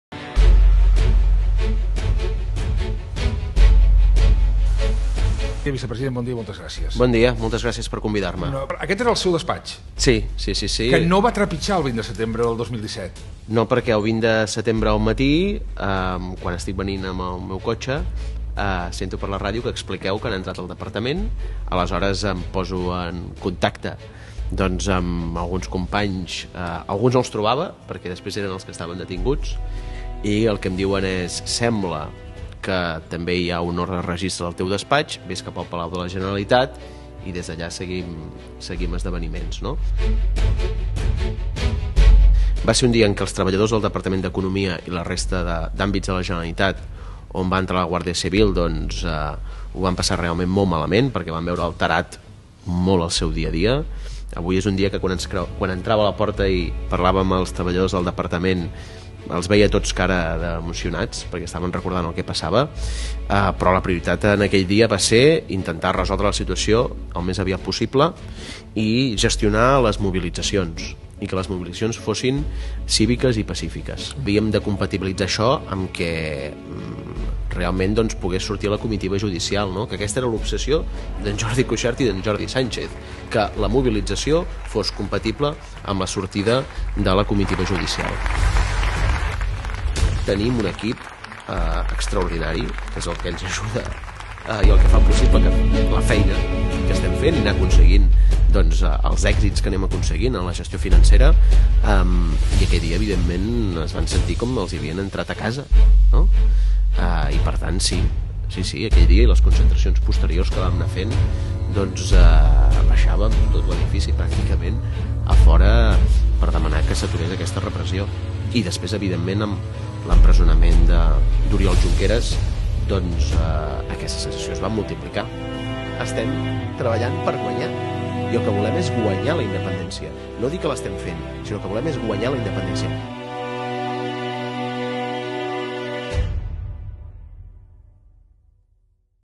Pere Aragonès: Ahir, un any després del 20-S, vam tornar a sortir 👫 als carrers
Pere Aragonès: Ahir, un any després del 20-S, vam tornar a sortir 👫 als carrers amb la força que hem demostrat sempre i més convençuts que mai que guanyarem la llibertat! TossudadamentAlçats SensePor Dempeus per construir la RepúblicaCatalana JuntsHoPodemTot @elvellblues